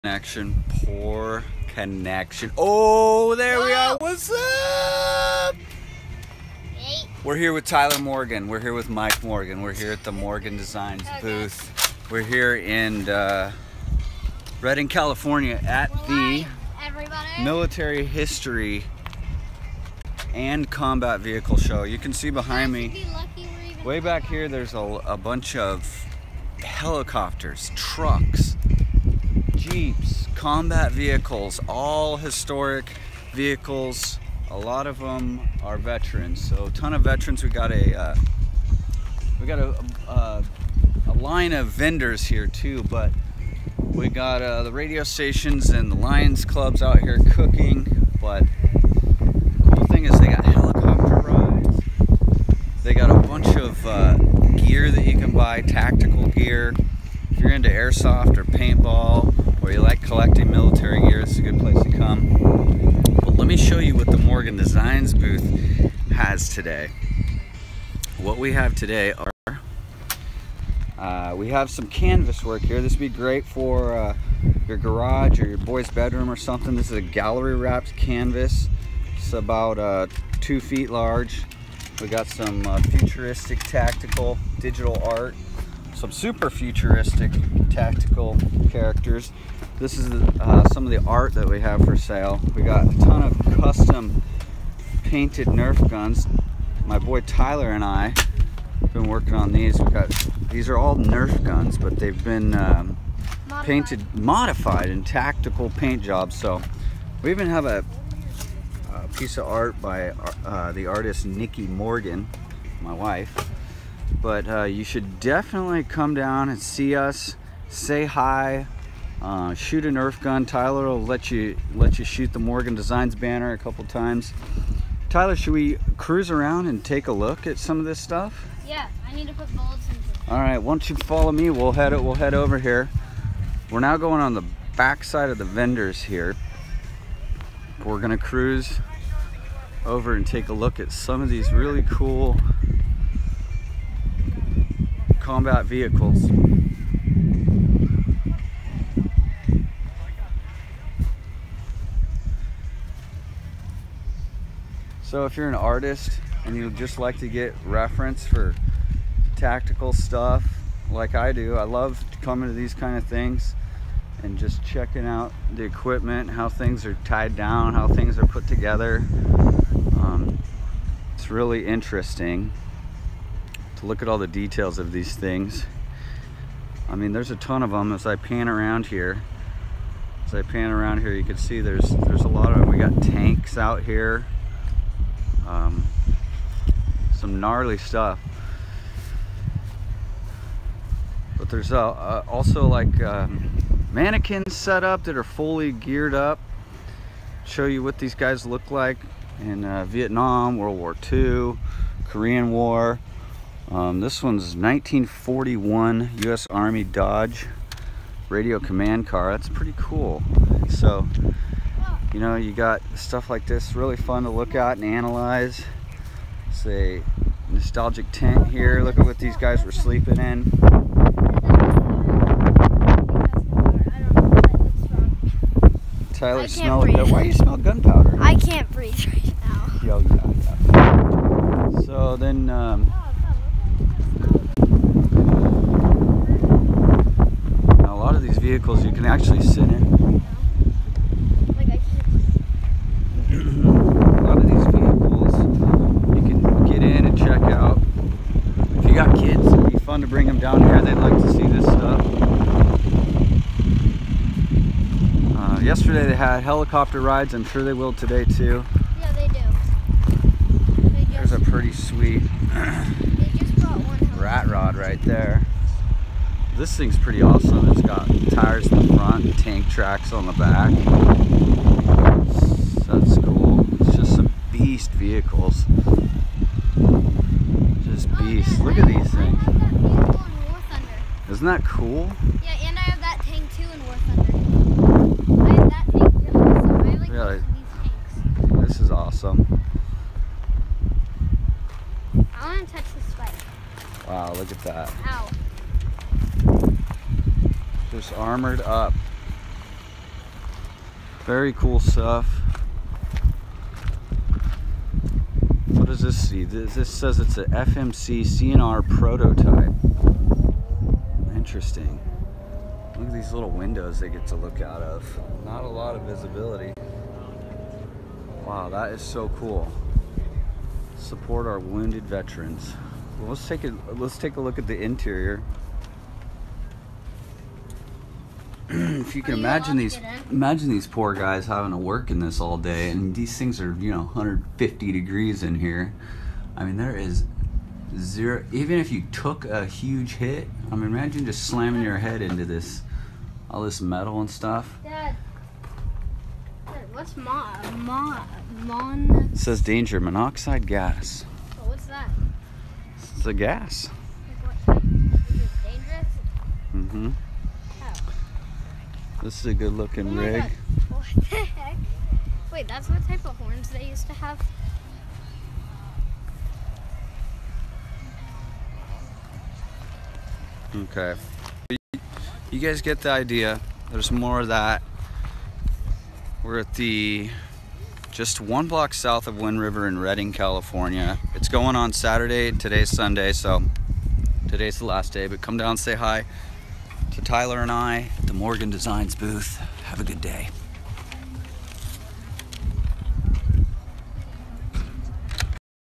Military history & Combat Vehicle Show with the Morgan Designs booth
This is a video introducing the Morgan Designs booth and a general walk-through showing some of the vehicles on display September 24, 2017 for the military combat vehicle show in Redding California